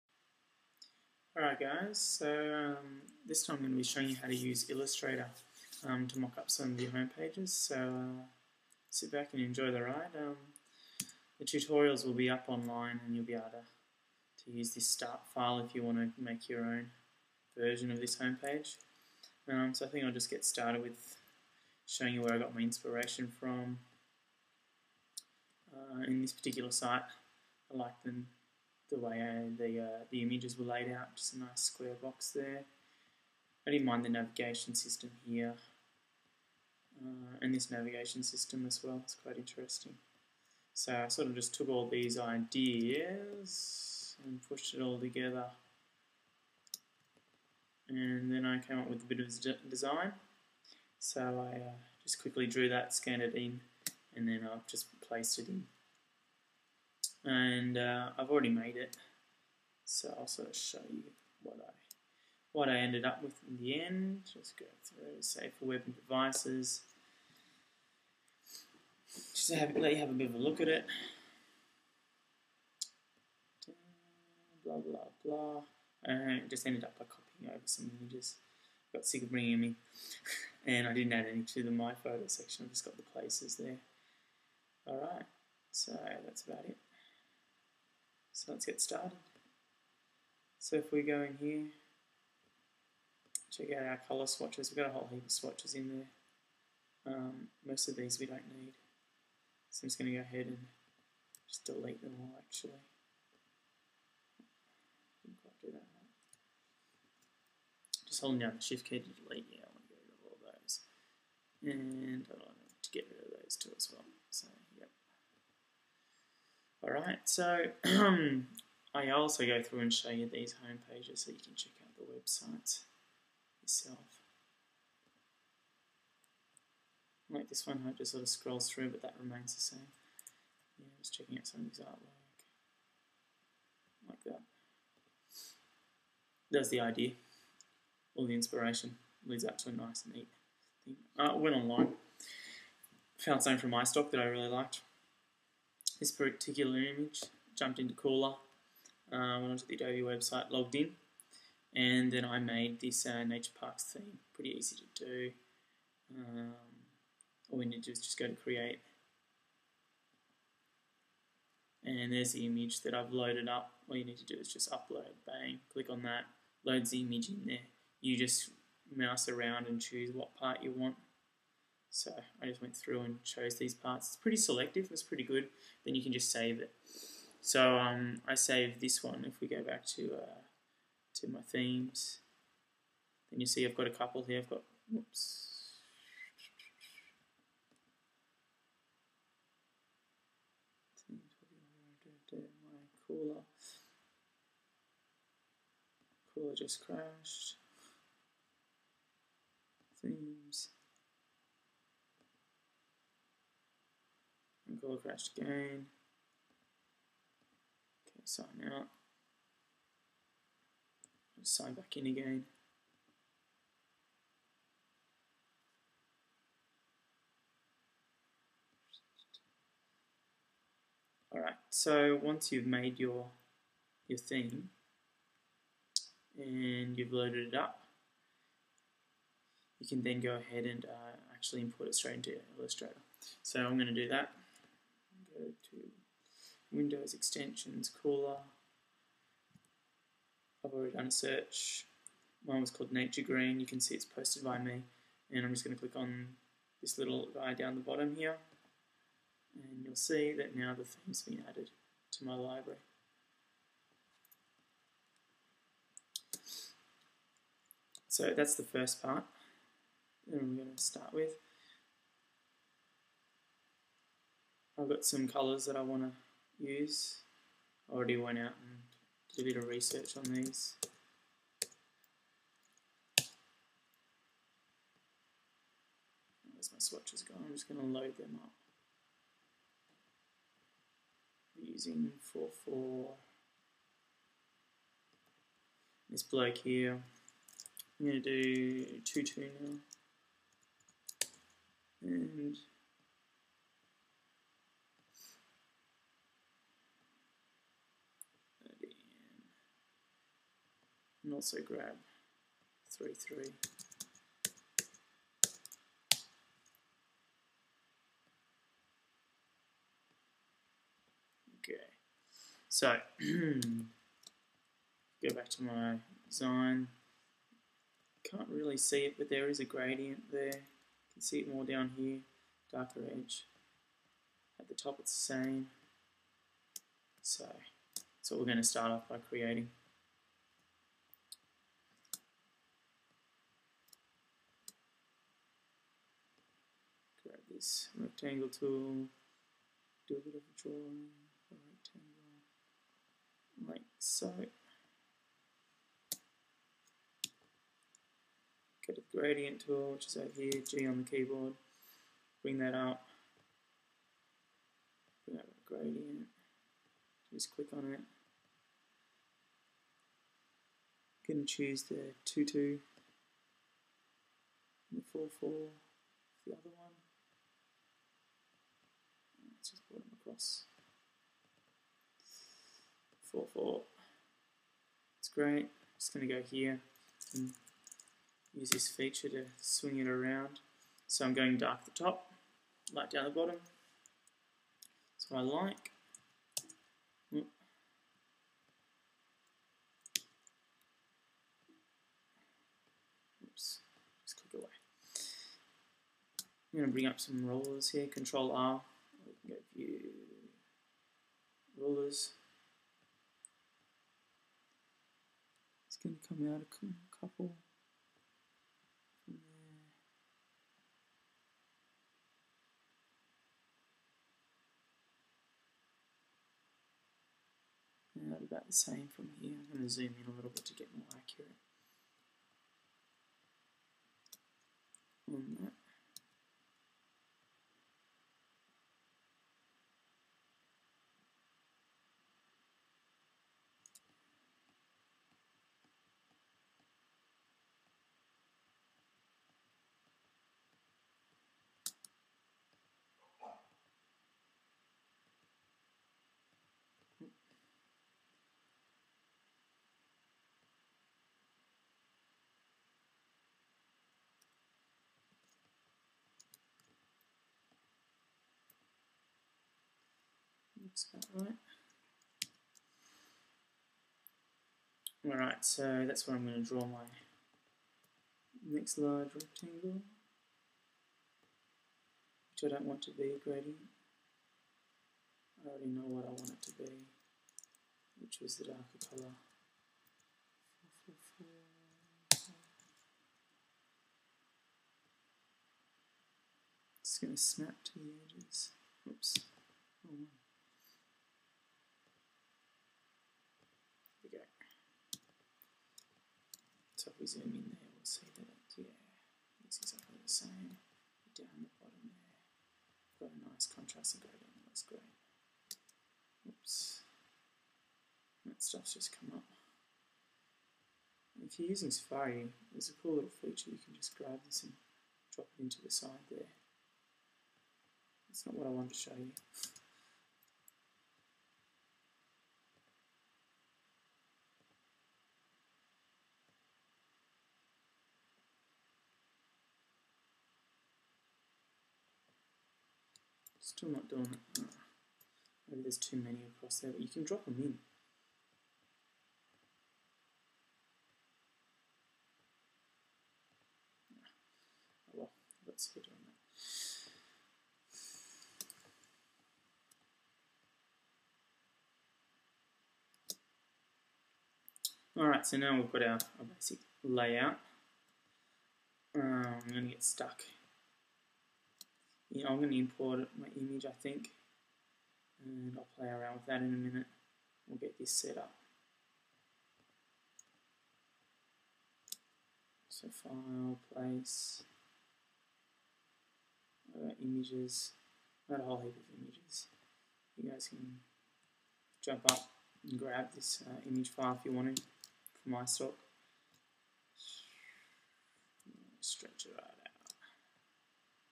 web2.0 Part1 Adobe Illustrator CS4
Create a website layout in Adobe Illustrator CS4